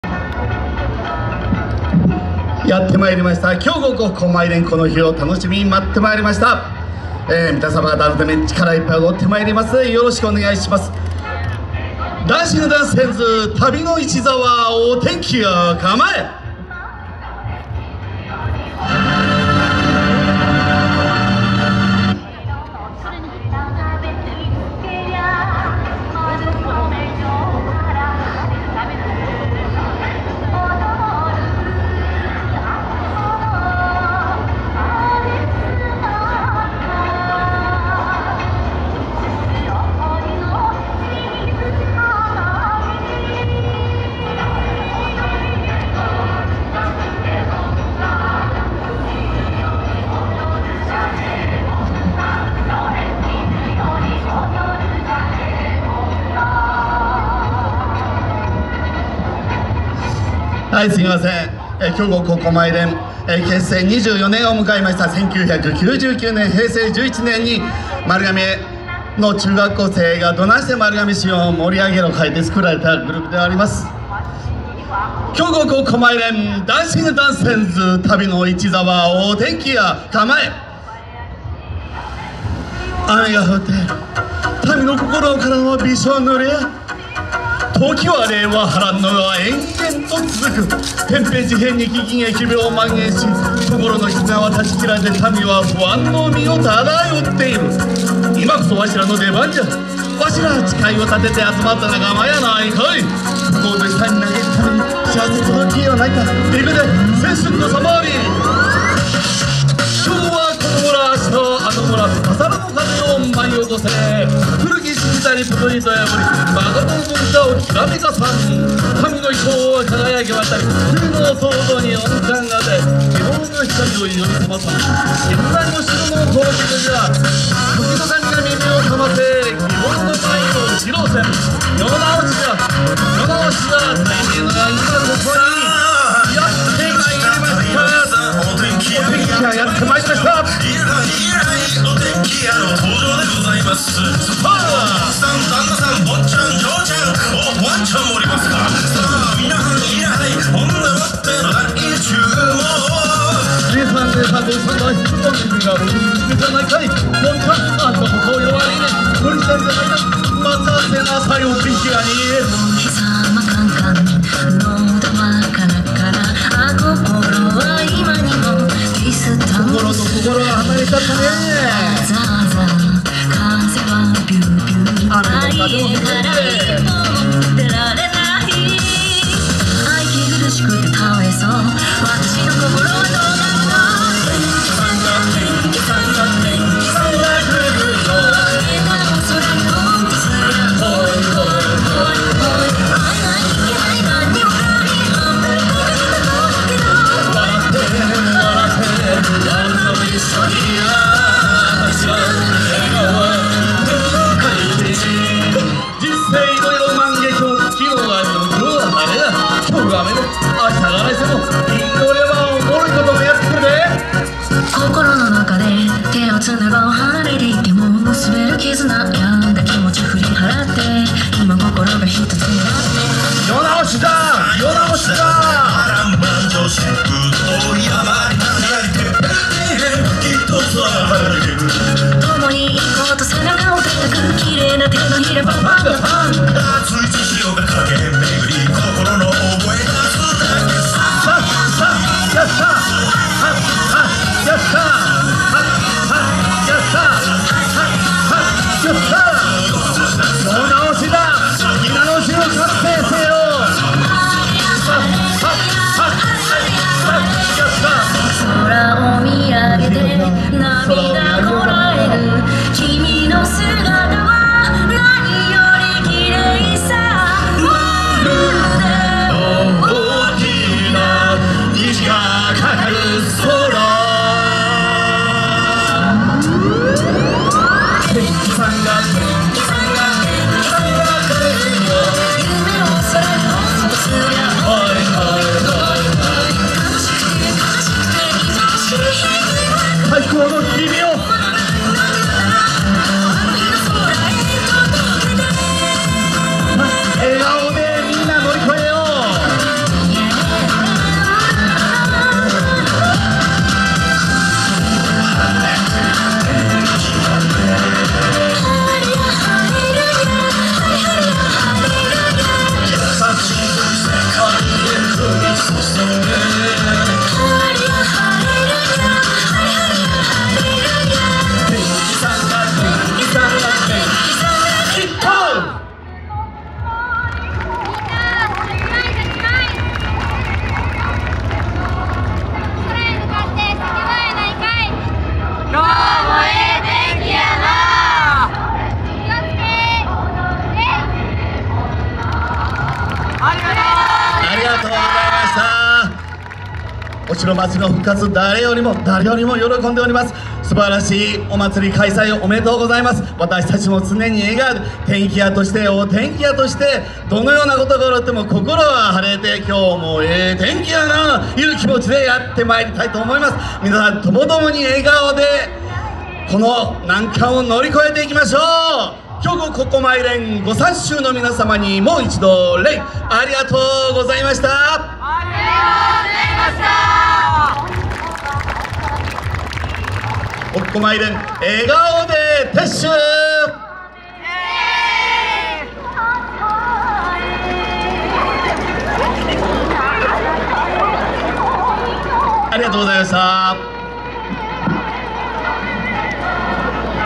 [4K] 丸亀市立南中学校ボランティア部 京極発幸舞連 丸亀お城まつり 2023 (中央)
丸亀お城まつり
2023年5月3日(水)
お堀端ステージ